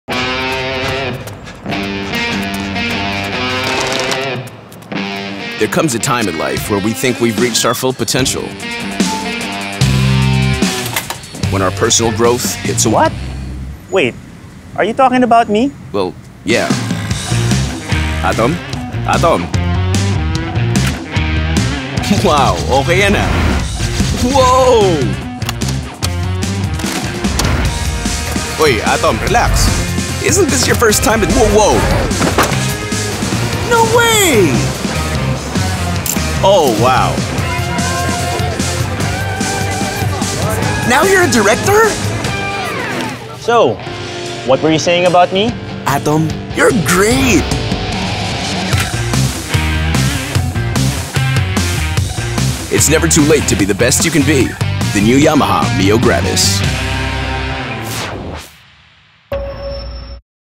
The Yamaha Mio Gravis
Be whoever you want to be with Yamaha’s newest AT- the Mio Gravis 👊

SRP PHP 85,900

The Yamaha Mio Gravis is available in Matte Red, Matte Green and Matte Black.

ASC NO. Y013N112219M